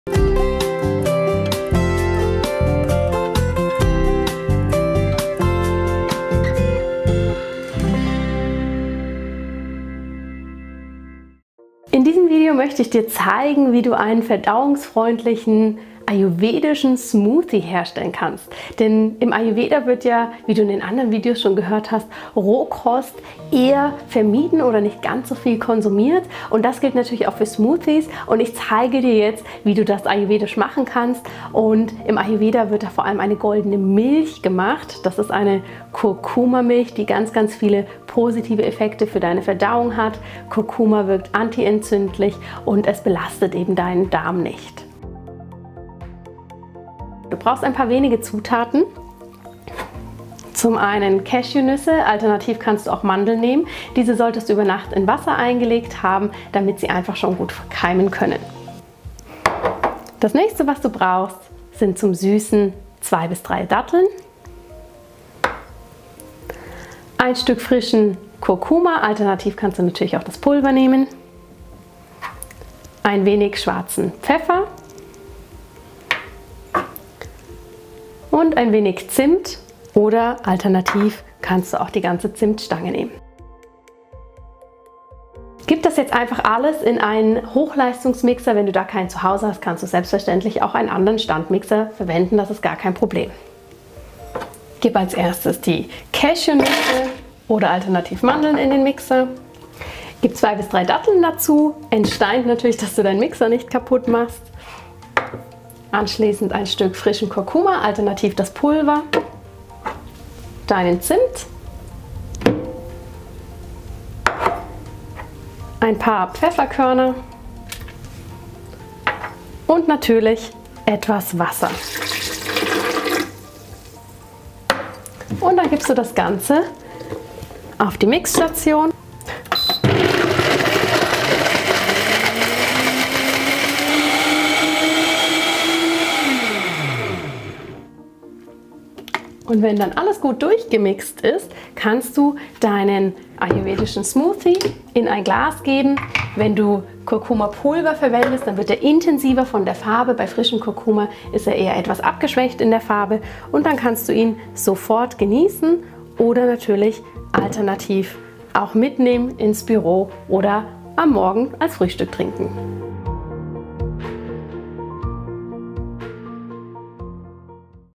Moderner Ayurveda mit Dr. Janna Scharfenberg #12 | Verdauung | Goldene Milch
Ayurveda ist ein ganzheitliches Gesundheitssystem aus Indien und wird dort schon seit vielen Generationen angewendet um Körper, Geist und Seele in Balance zu bringen. Die präventive Medizin des Langen Lebens besteht aus vielen Komponenten und kann auch im Alltag angewendet werden. Dr. Janna Scharfenberg gibt dir Tipps, wie auch du von den Vorzügen des Ayurveda profitieren kannst.
Hier findest du das ayurvedische Rezept für die "Goldene Milch".